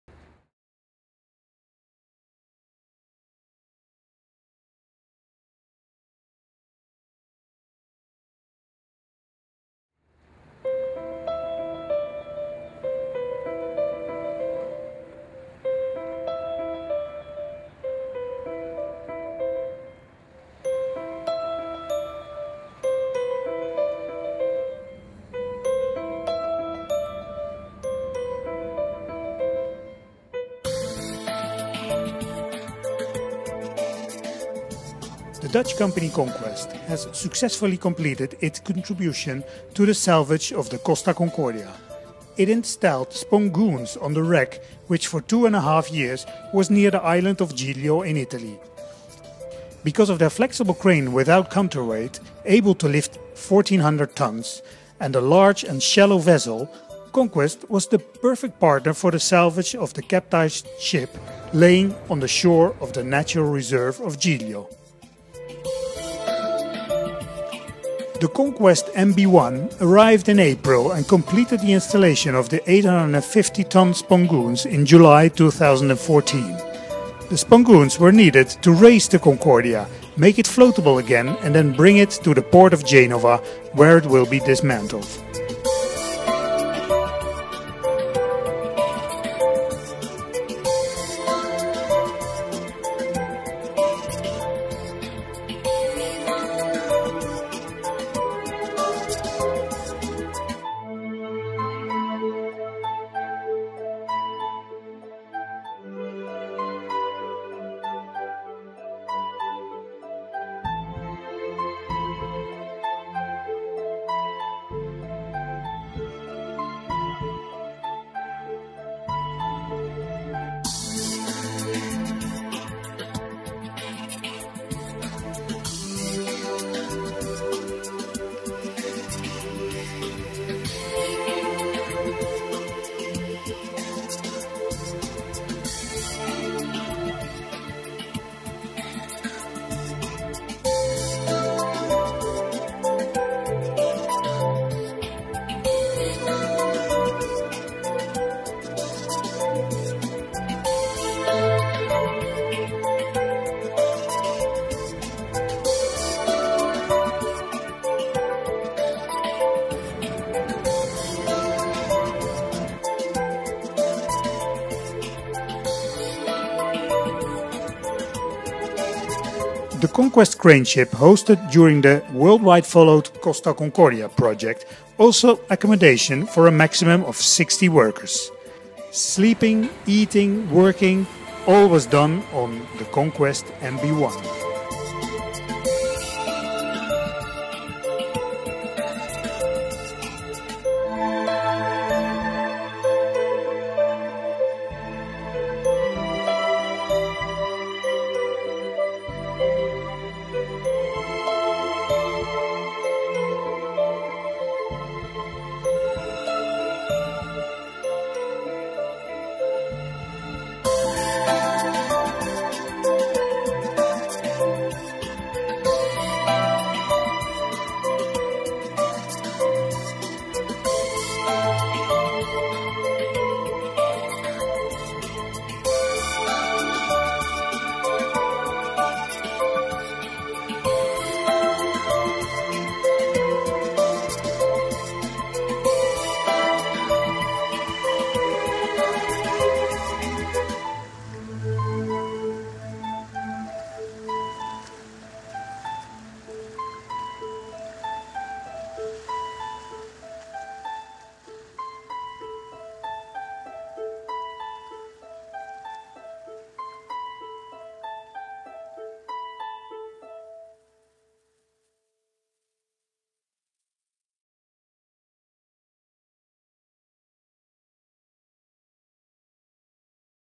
Conquest MB1 installing spongoons at Costa Concordia
Heavy lift crane barge Conquest MB1 installing spongoons alongside the Costa Concordia to get her floating again.